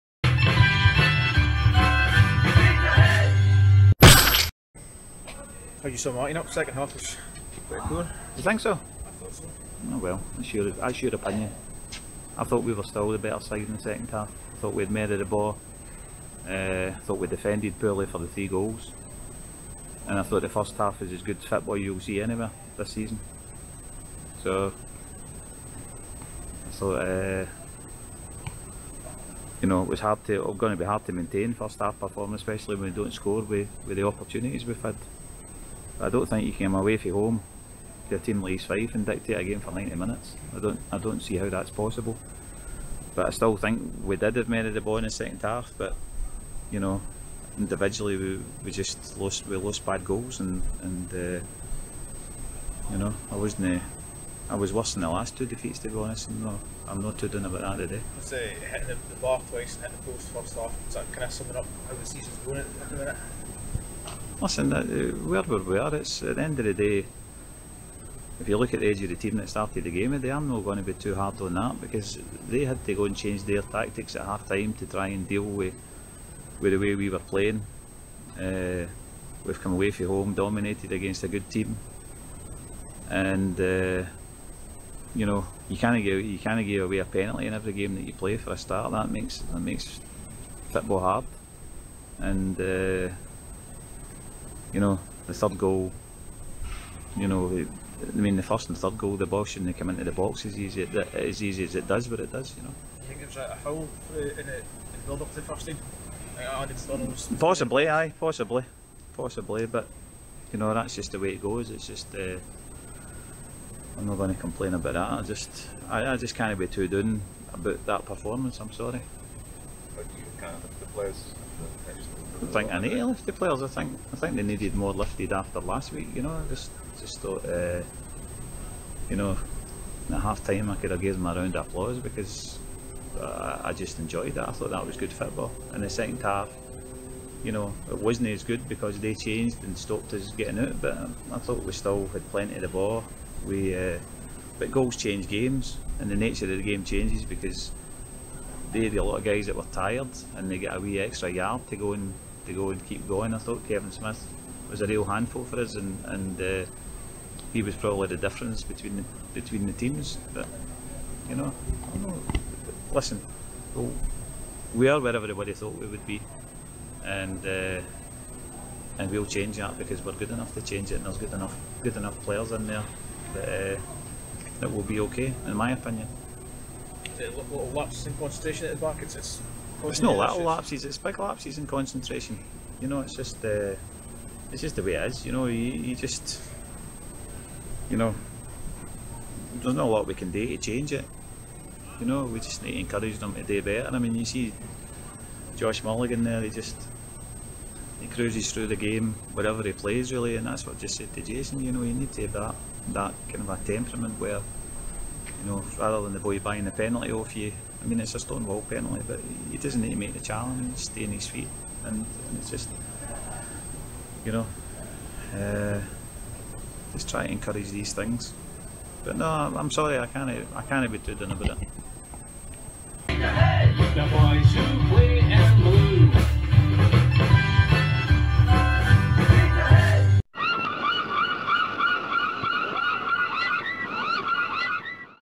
JMPOSTMATCHEASTFIFE280821
Peterhead FC manager Jim McInally gives hius thoughts after the 3-0 loss to East Fife on 28/08/21